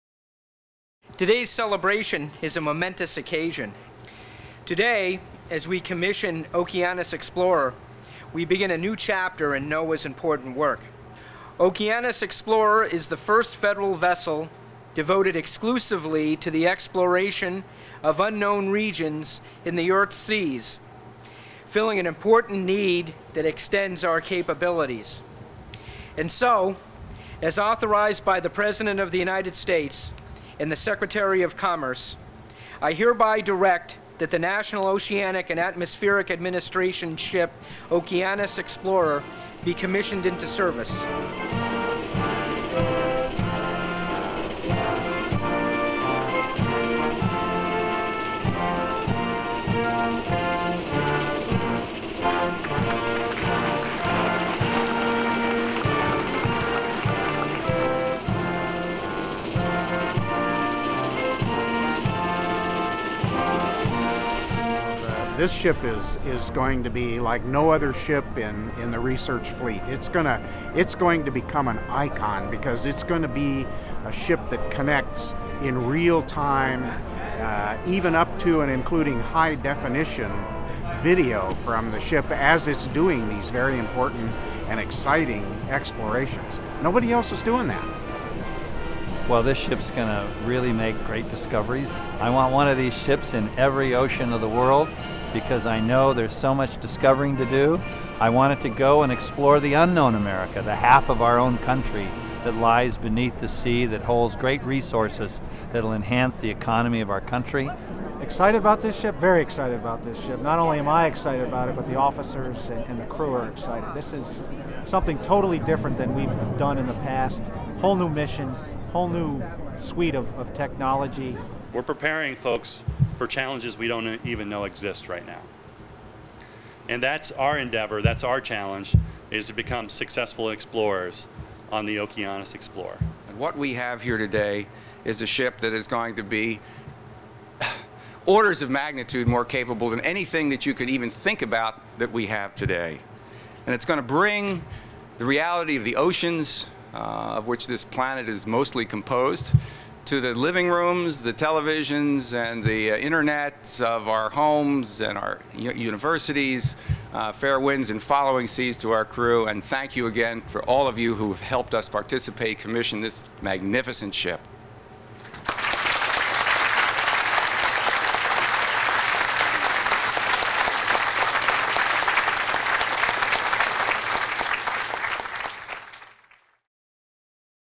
Commissioning of NOAA Ship OKEANOS EXPLORER
On August, 13, 2008 in Seattle, WA, the NOAA Ship OKEANOS EXPLORER, "America's Ship for Ocean Exploration," was commissioned into service.  This video shows highlights from the commissioning ceremony.  Watch to hear what some distinguished guests had to say, including Deputy Secretary of Commerce, John J. Sullivan, Vice Admiral Conrad C. Lautenbacher, Jr., Dr. Stephen R. Hammond, Dr. Robert D. Ballard, Rear Admiral Jonathan W. Bailey, and first commanding officer of the OKEANOS EXPLORER, Commander Joseph A. Pica.